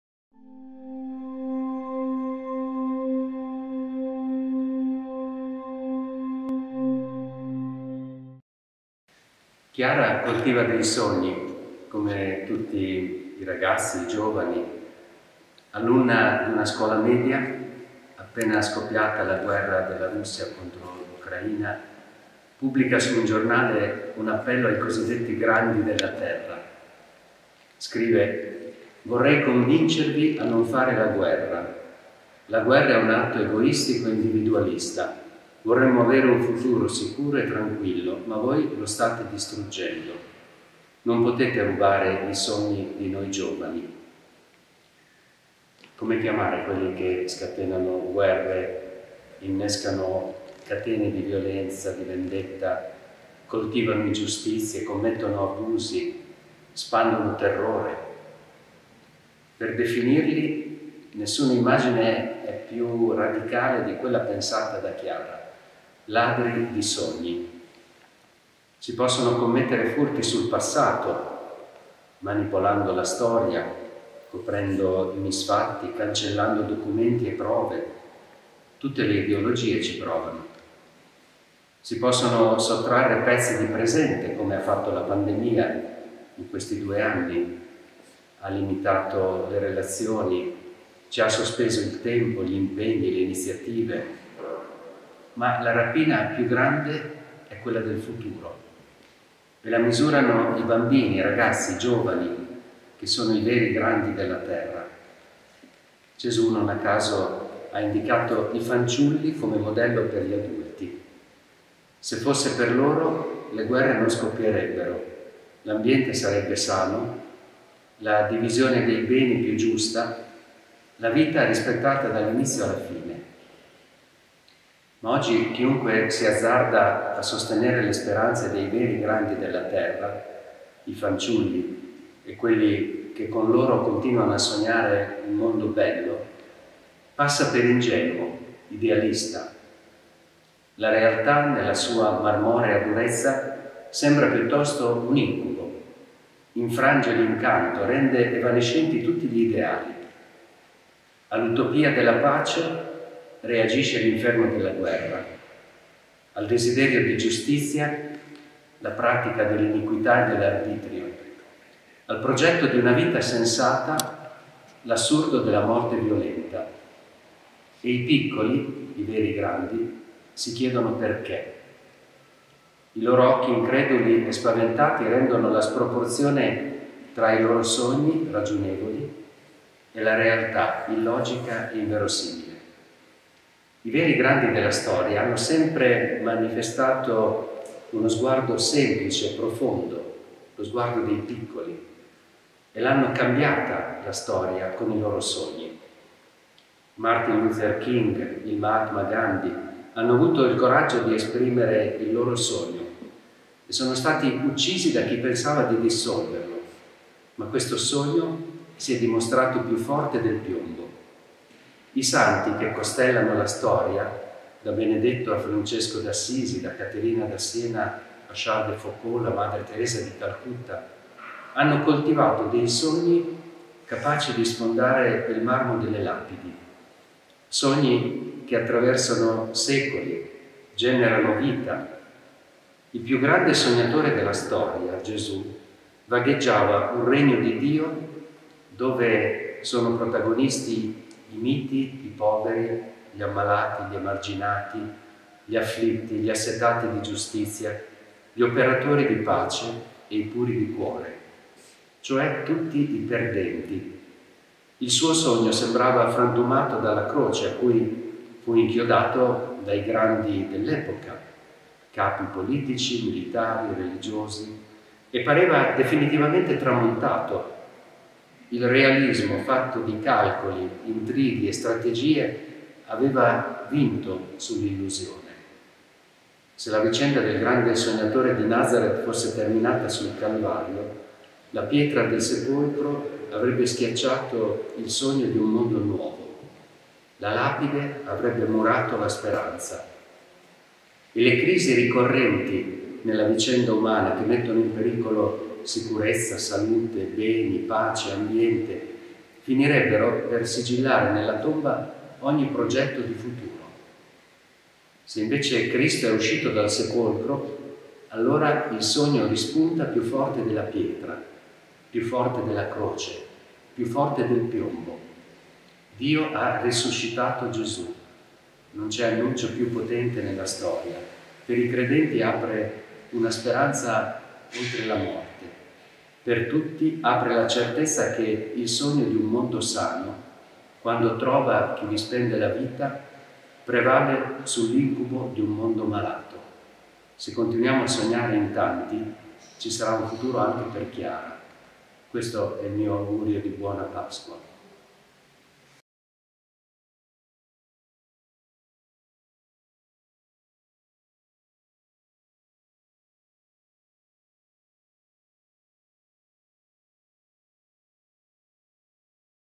Messaggio Pasquale 2022
Messaggio Pasquale del Vescovo Erio Castellucci  alla Arcidiocesi di Modena   Nonantola e Diocesi di Carpi